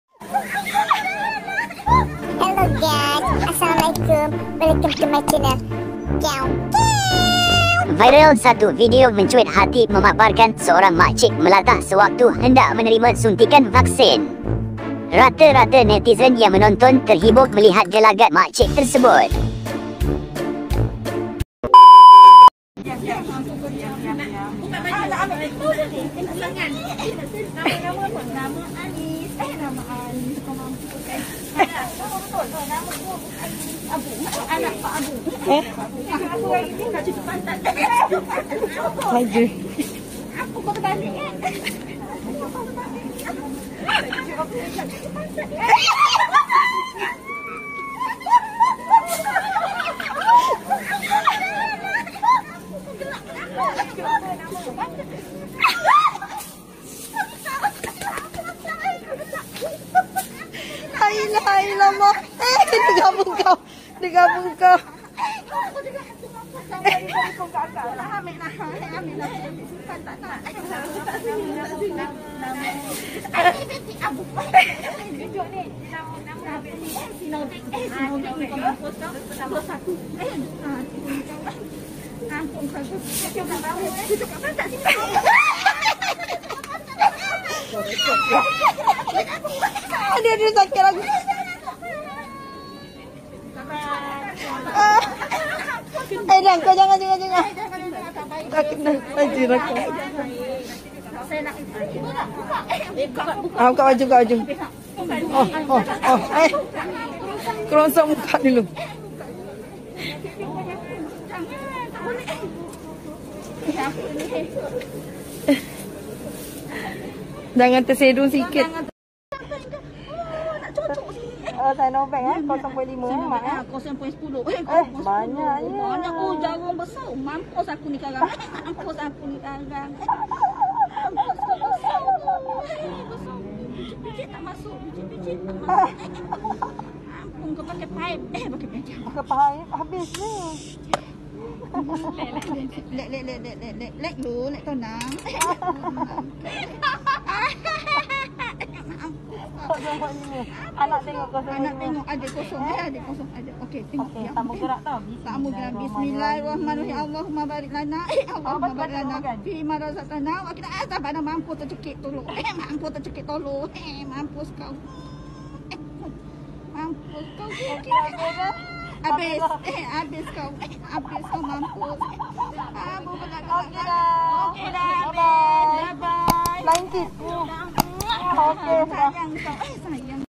Gelagat Wanita Melatah Masa Cucuk Vaksin Buat Netizen Berdekah
Viral satu video memaparkan seorang wanita melatah di pusat vaksin sewaktu hendak menerima suntikan.